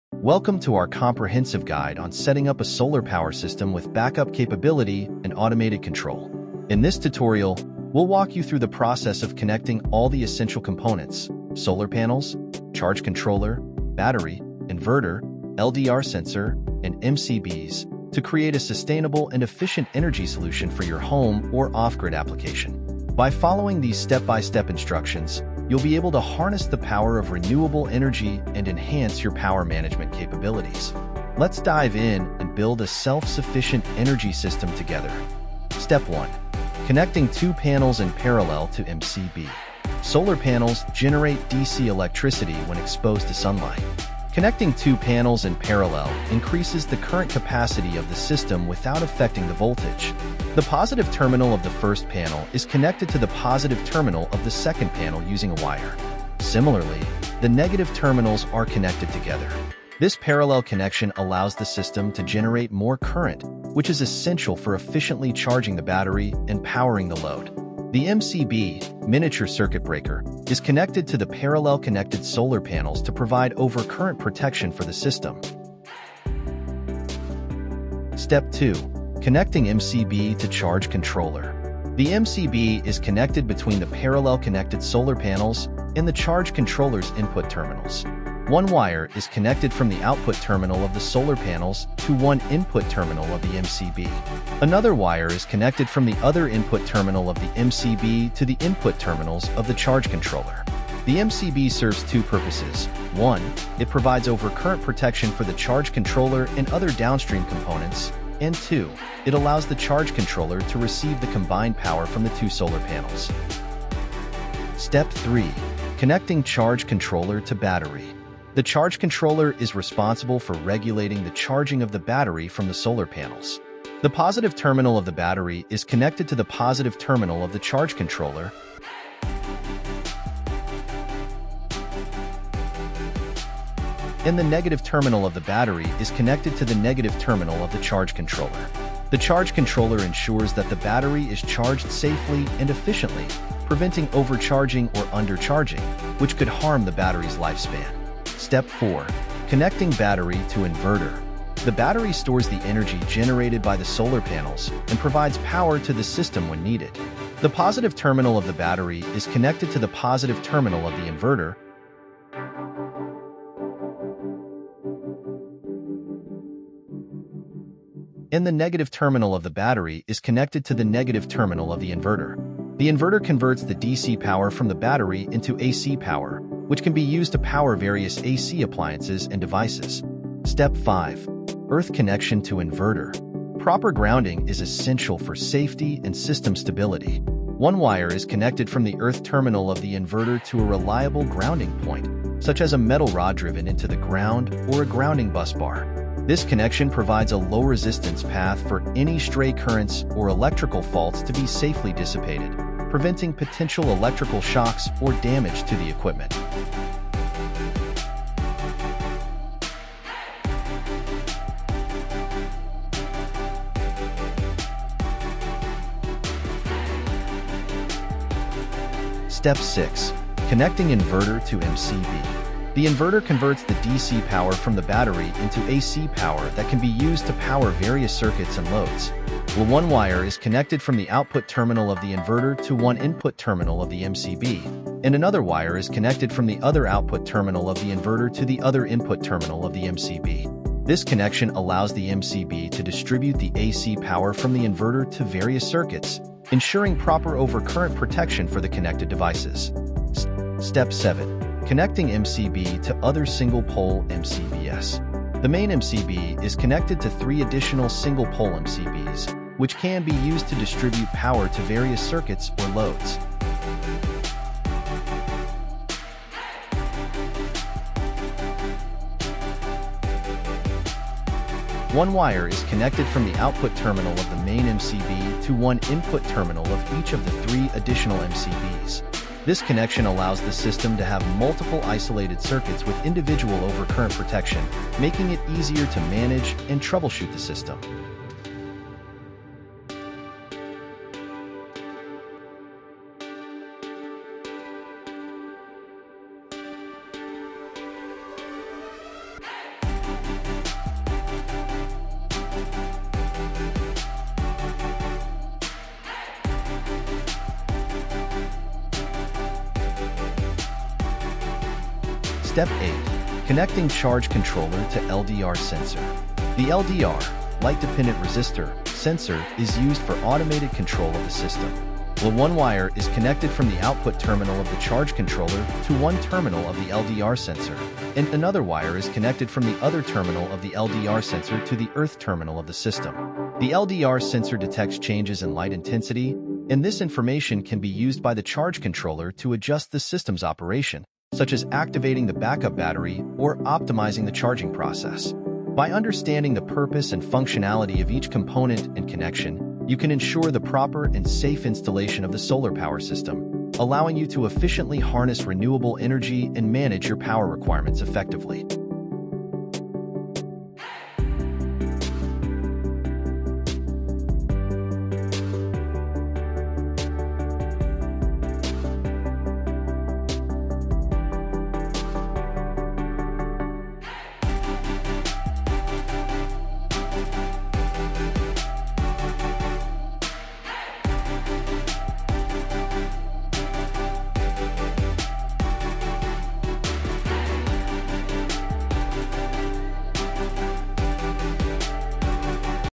The ULTIMATE Solar Panel And LDR Wiring Guide (DIY)
In this step-by-step tutorial, we'll guide you through the process of setting up a comprehensive solar power system that includes backup capability and automated control. Whether you're looking to power your home, a remote cabin, or an off-grid application, this tutorial will provide you with the knowledge and instructions to harness renewable energy efficiently.

We'll cover the following key components and their interconnections:
- Solar Panels: Learn how to connect multiple solar panels in parallel to increase the current capacity.
- Charge Controller: Understand the role of the charge controller in regulating the battery charging from the solar panels.
- Battery: Discover how to properly connect the battery to the charge controller and inverter for reliable power storage.
- Inverter: See how the inverter converts the DC power from the battery into AC power for your household appliances and devices.
- LDR Sensor: Explore the use of a Light Dependent Resistor (LDR) sensor for automated control of the solar power system.
- Miniature Circuit Breakers (MCBs): Learn how to incorporate MCBs for overcurrent protection and circuit distribution.

By following this comprehensive tutorial, you'll be able to set up a sustainable and self-sufficient energy solution for your home or off-grid project. Don't miss this opportunity to take control of your power needs and contribute to a greener future!